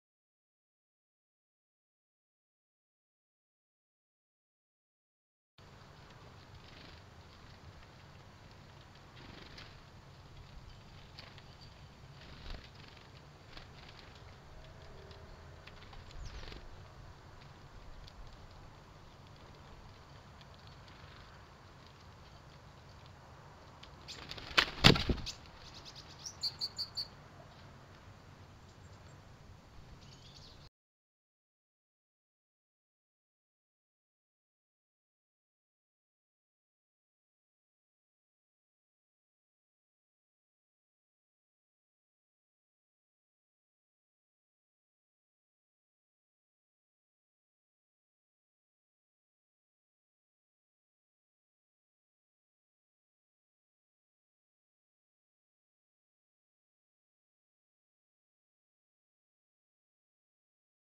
L'épervier attrape une mésange - 27/02/2023 - Le Jardin de Théding
Découvrez l'intégralité de la saison 2022-23 des oiseaux de Théding aux mangeoires sur cette
Aussi à l'automne/hiver, suivez les mangeoires du jardin en direct